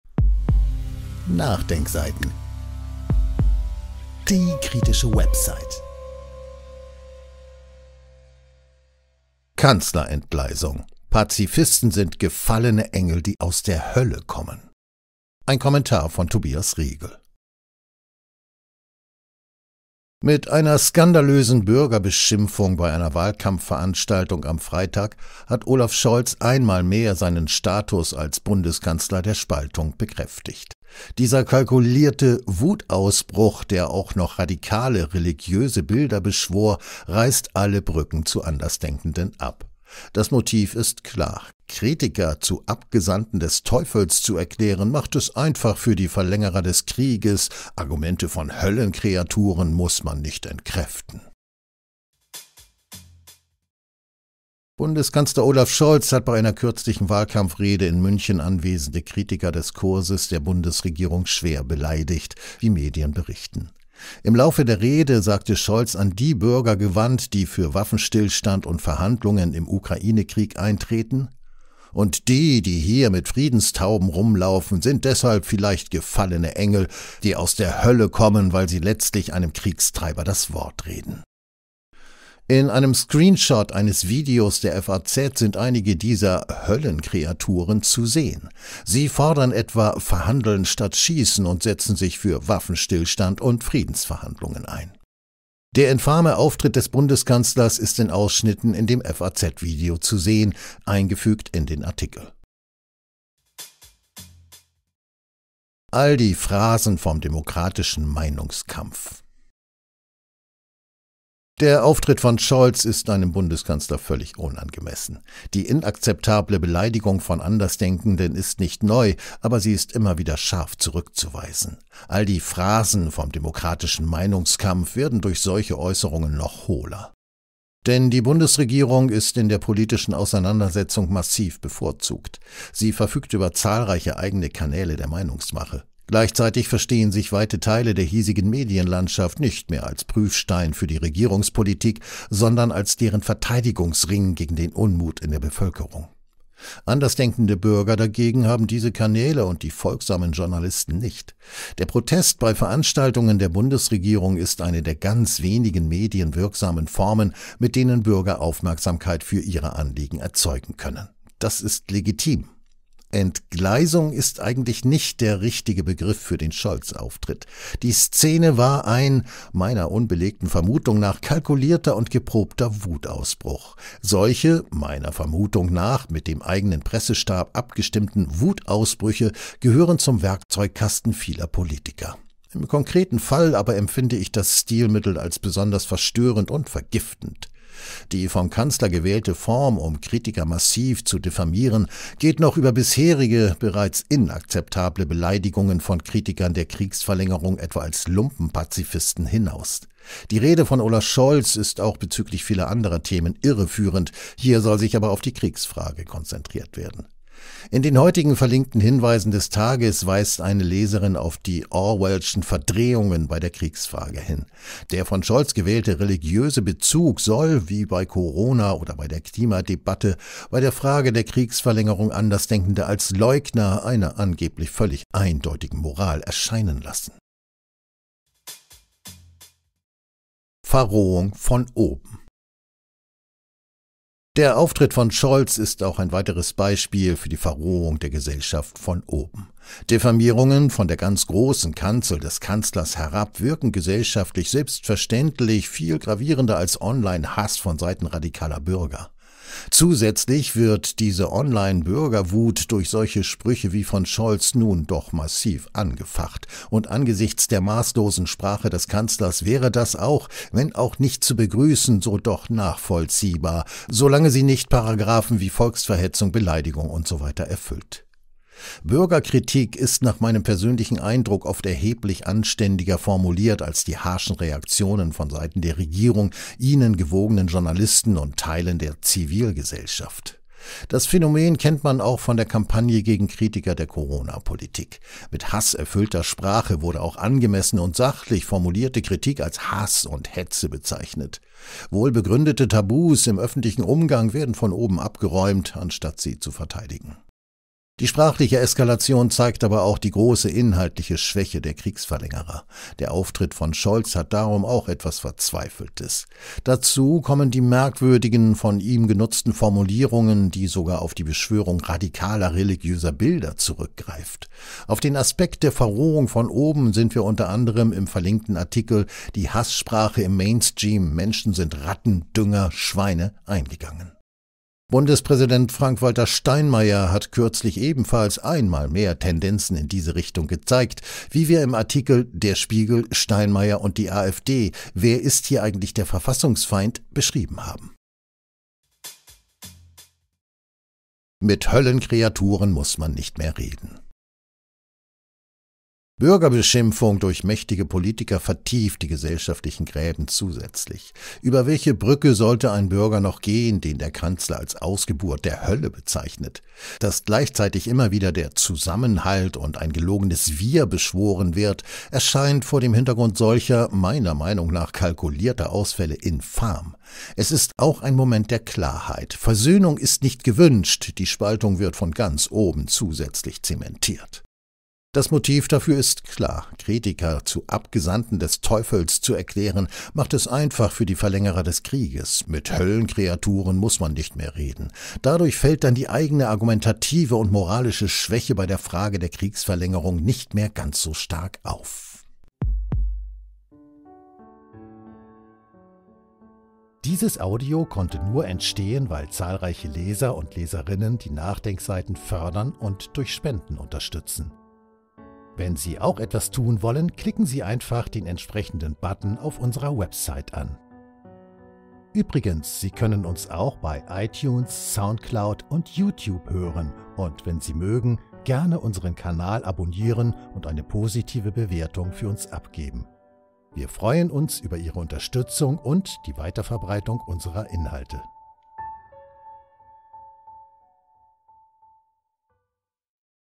Kanzler-Entgleisung: Pazifisten sind „gefallene Engel, die aus der Hölle kommen“ | Tobias Riegel NDS
Kanzler-Entgleisung: Pazifisten sind „gefallene Engel, die aus der Hölle kommen“ | Tobias Riegel | NachDenkSeiten-Podcast | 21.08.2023

Mit einer skandalösen Bürgerbeschimpfung bei einer Wahlkampfveranstaltung am Freitag hat Olaf Scholz einmal mehr seinen Status als Bundeskanzler der Spaltung bekräftigt: Dieser kalkulierte „Wutausbruch“, der auch noch radikale religiöse Bilder beschwor, reißt alle Brücken zu Andersdenkenden ab. Das Motiv ist klar: Kritiker zu Abgesandten des Teufels zu erklären, macht es einfach für die Verlängerer des Krieges – Argumente von Höllen-Kreaturen muss man nicht entkräften. Ein Kommentar von Tobias Riegel.

Artikel veröffentlicht am: 21. August 2023 um 10:00

einzelne Politiker, Personen der Zeitgeschichte, Kampagnen, Tarnworte, Neusprech, Militäreinsätze, Kriege,
Diffamierung, diplomatische Verhandlungen, hate speech, Pazifismus, Olaf Scholz, Verrohung